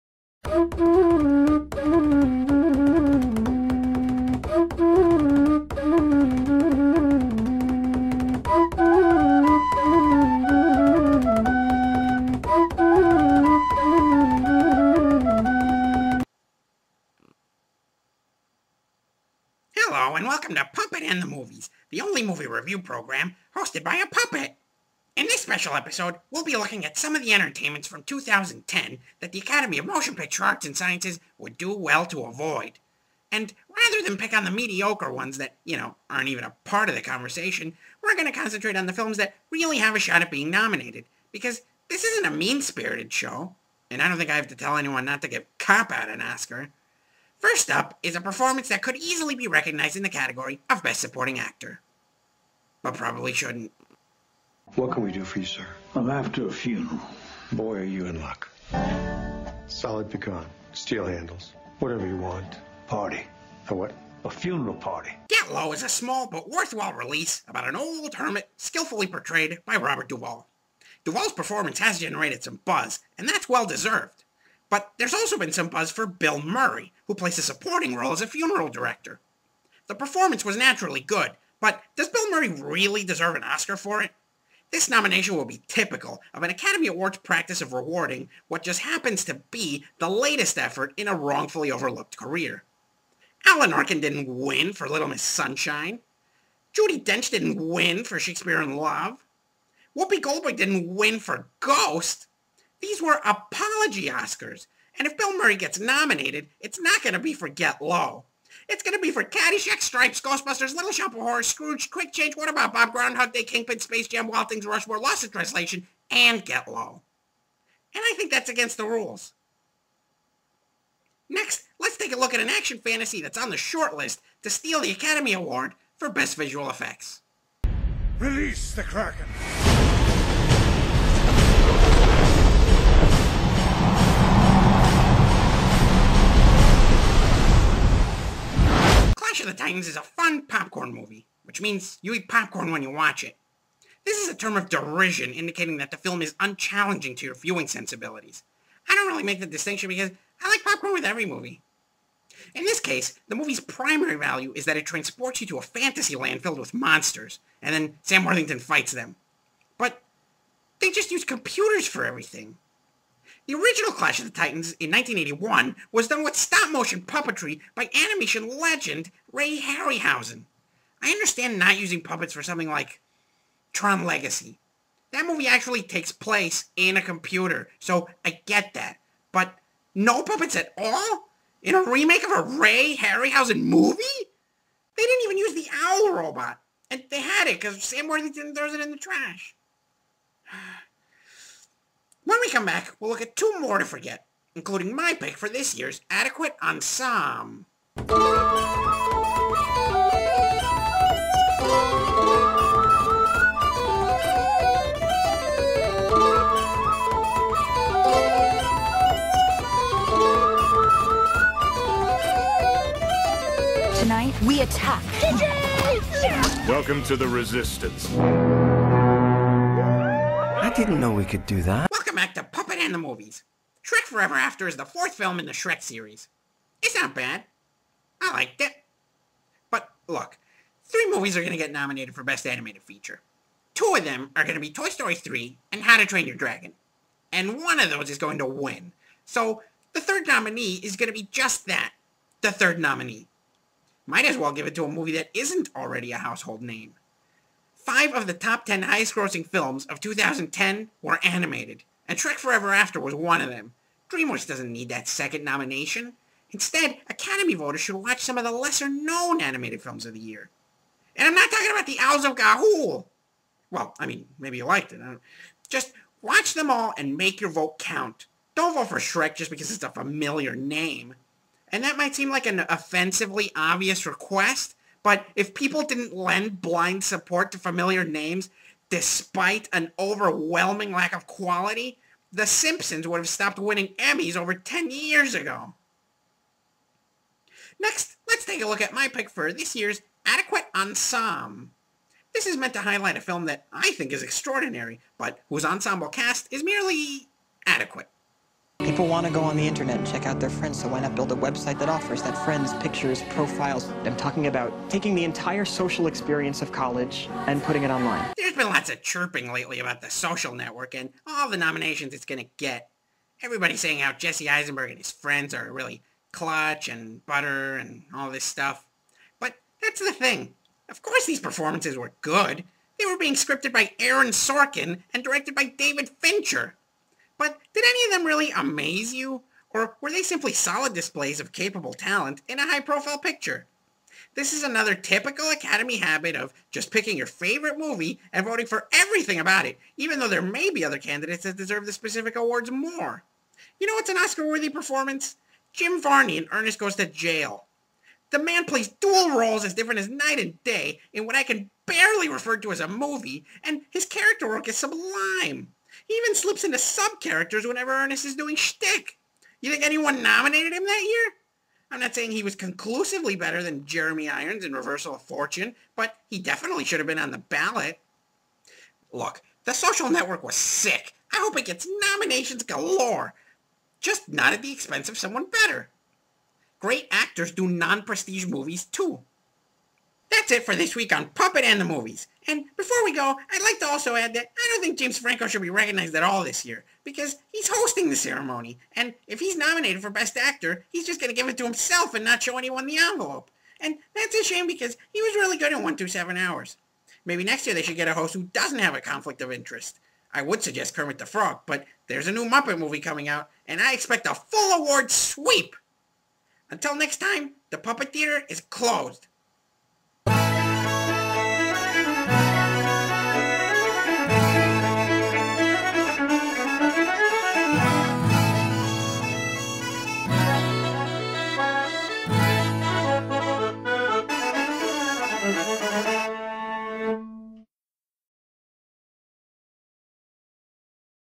Puppet & The Movies -- "Hey, Academy!" Four to Forget
In this special edition of Puppet & The Movies, Puppet raises concerns about four potential Academy Award nominees.